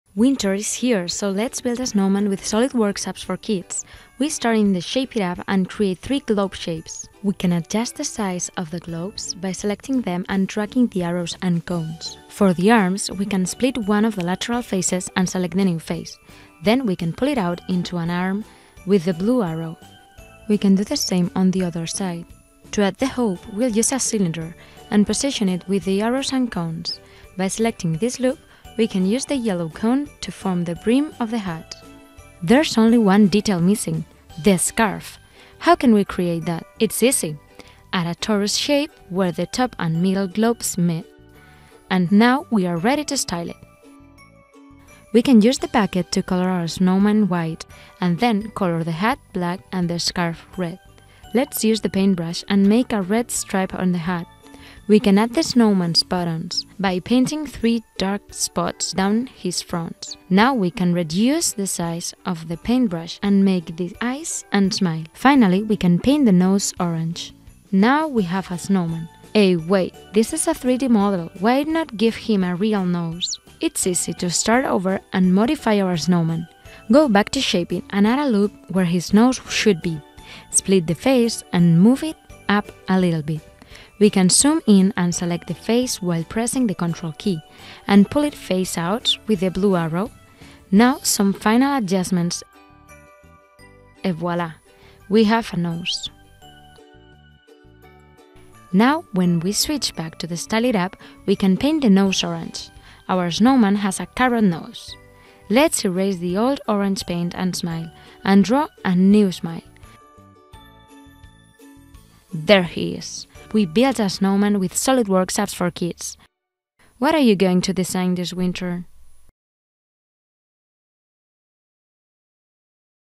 Apps for Kids - Let's Build a Snowman - SOLIDWORKS
Apps for Kids - Let's Build a Snowman - SOLIDWORKS. Learn how to build a snowman in SOLIDWORKS Apps for Kids.